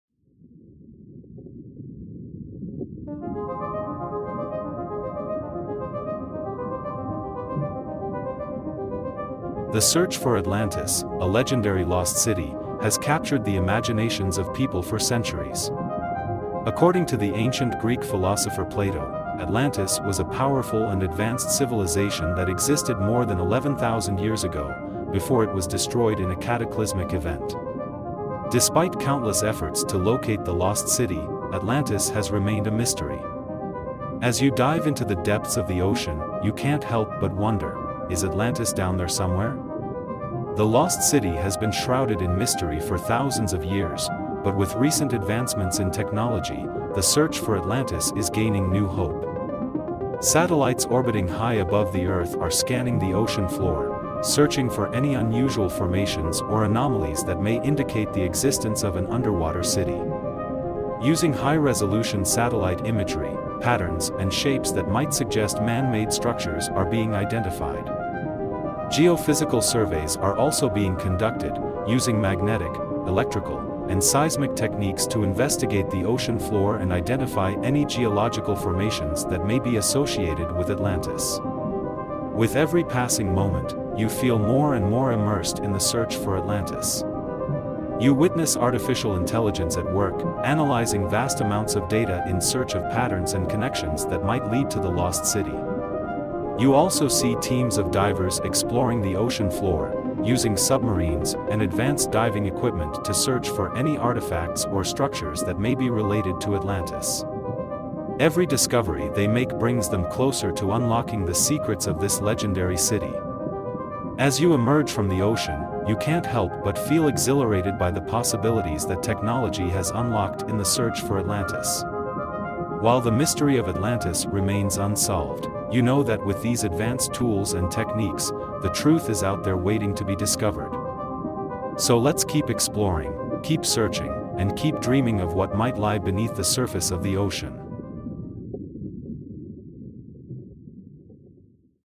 Atlantis Revealed: Latest Technology Found the Lost City
Join us on a journey to discover one of the greatest mysteries of human history - the lost city of Atlantis. According to ancient Greek mythology, Atlantis was a powerful and technologically advanced civilization that was destroyed in a cataclysmic event. While there is no conclusive evidence that Atlantis ever existed, the search for this legendary city has captured the imaginations of people for centuries. In this video, we explore how technology is being used to search the ocean floor and unlock the secrets of Atlantis. From satellite imaging and geophysical surveys to autonomous underwater vehicles and artificial intelligence, we take a closer look at the cutting-edge tools and techniques that are being used to search for Atlantis. So come with us as we dive into the depths of the ocean and embark on a quest to unlock the mysteries of this legendary lost city.